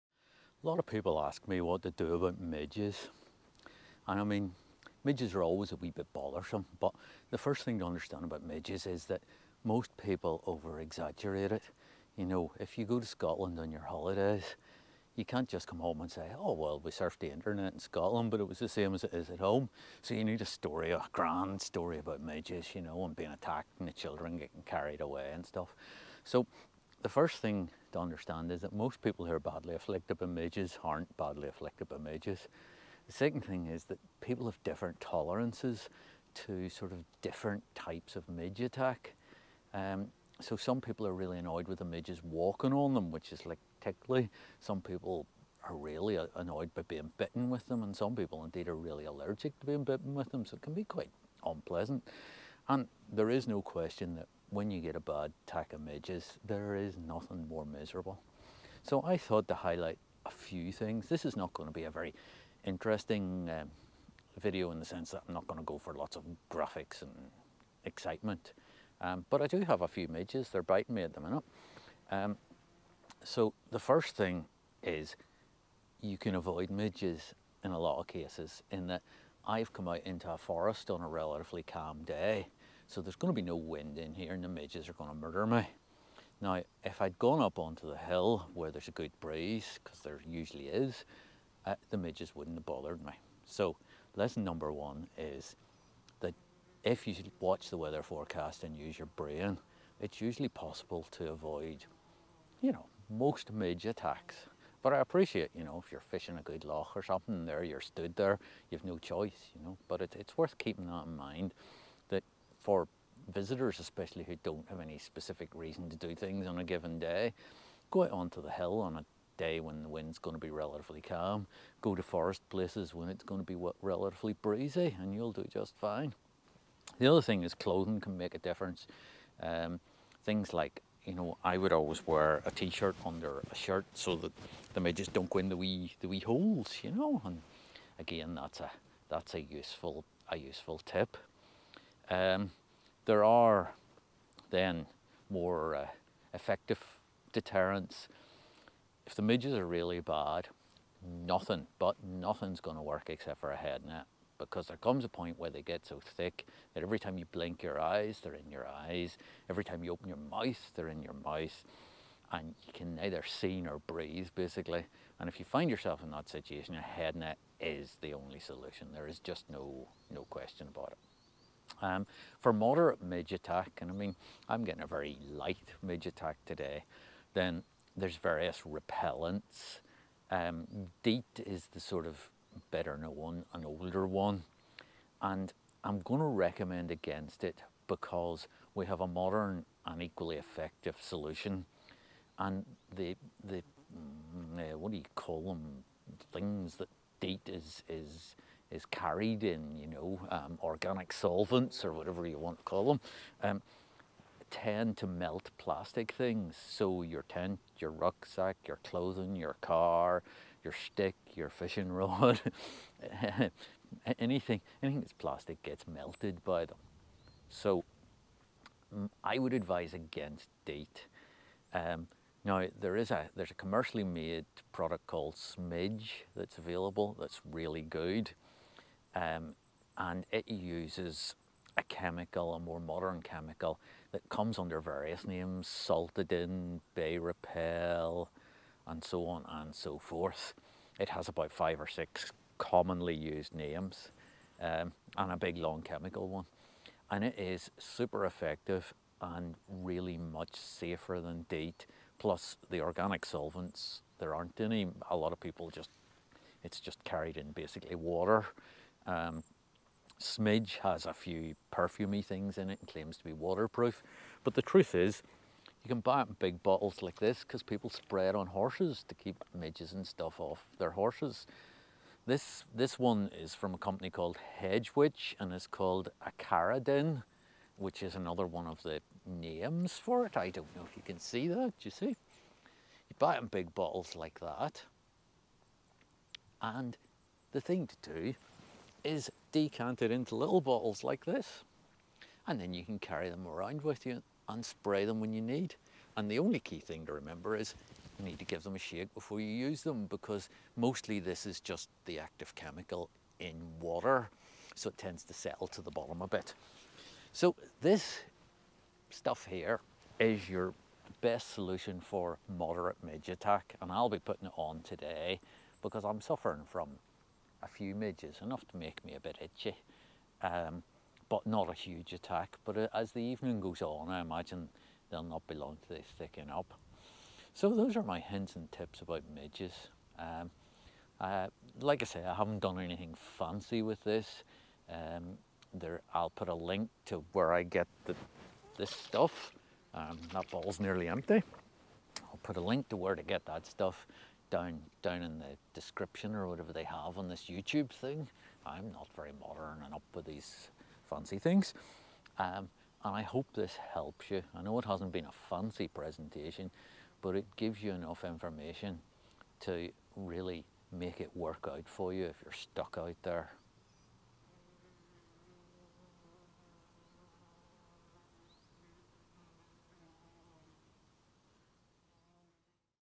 Defeating the Scottish midge - some hints and tips
There can be nothing as uncomfortable as a good attack of the midges and so I thought to pass on a few hints and tips that might help people. For some this will all seem like stating the obvious but for others you might get a few ideas.

If you just want to run into a shop and get some midge repellent then it is well worth trying Smidge as the chemical used is very effective and it will not damage plastic objects or similar. Smidge is perfumed, which I personally don't like, but they do claim that it is waterproof and while I've never seen any difference to other preparations this might make the difference for you:

As suggested in the video you can get large bottles of, basically, the raw chemical used in Smidge (and many other) repellents from the likes of Hedgewitch. This is exactly the same active ingredient basically dissolved in water with a bit of aloe vera so it doesn't have the fancy perfume etc. of some other preparations as it is designed to be used on horses. If you are out a lot you can decant this product into smaller spray bottles (you can get the off ebay) for ease of use in the field: